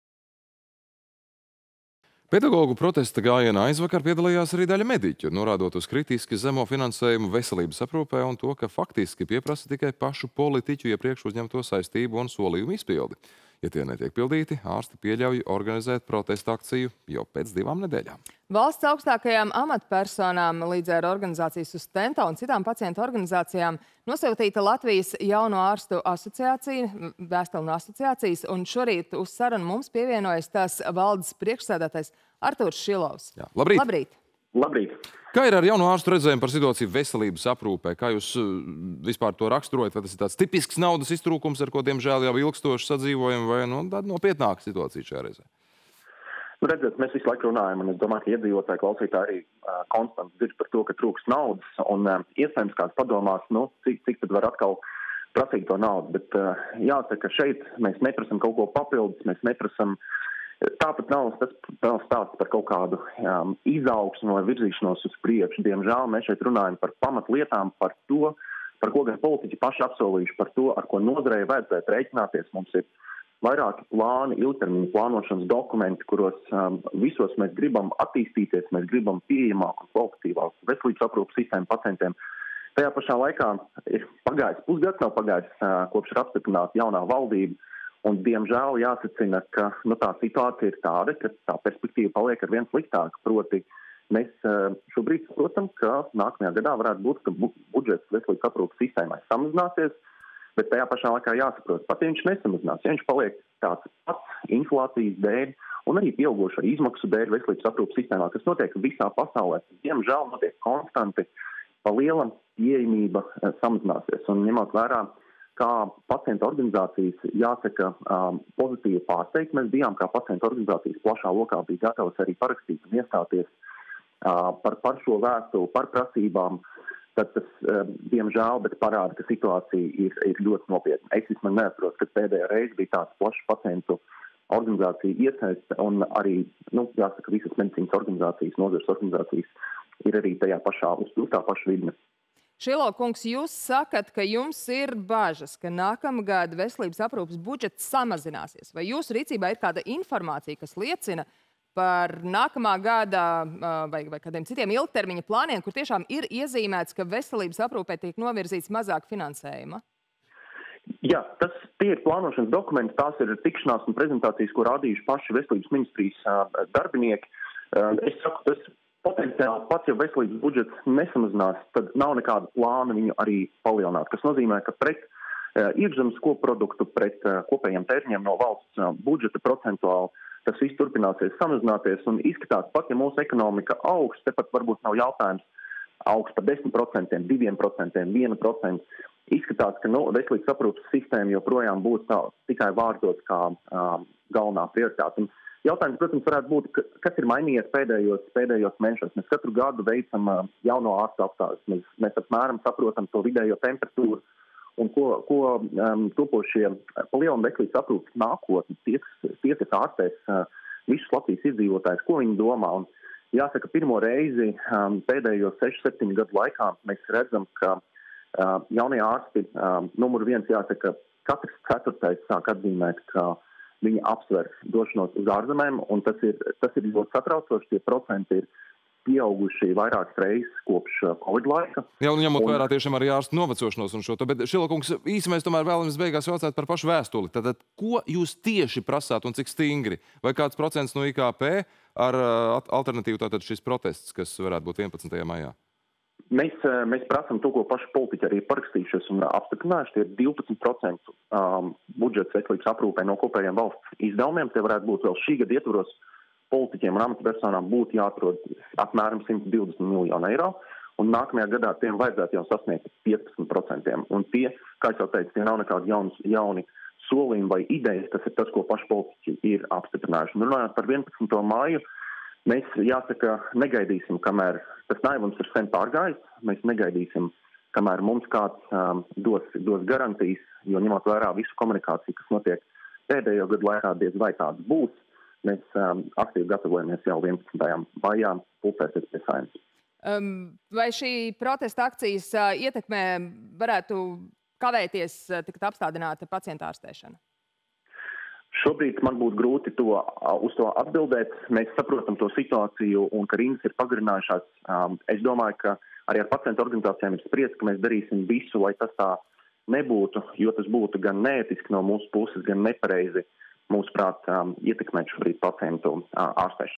Telefonintervija ar Latvijas Jauno ārstu asociācijas valdes priekšsēdētāju Artūru Šilovu
Pedagogu protesta gājienā aizvakar piedalījās arī daļa mediķu, norādot uz kritiski zemo finansējumu veselības aprūpē un to, ka faktiski pieprasa tikai pašu politiķu iepriekš uzņemto saistību un solījumu izpildi. Ja tie netiek pildīti, ārsti pieļauj organizēt protesta akciju jau pēc 2 nedēļām.
Vēstuli valsts augstākajām amatpersonām (līdz ar organizāciju SUSTENTO un citām pacientu organizācijām) nosūtījusi Latvijas Jauno Ārstu asociācija. Šorīt uz sarunu mums pievienojas tās valdes priekšsēdētājs Artūrs Šilovs.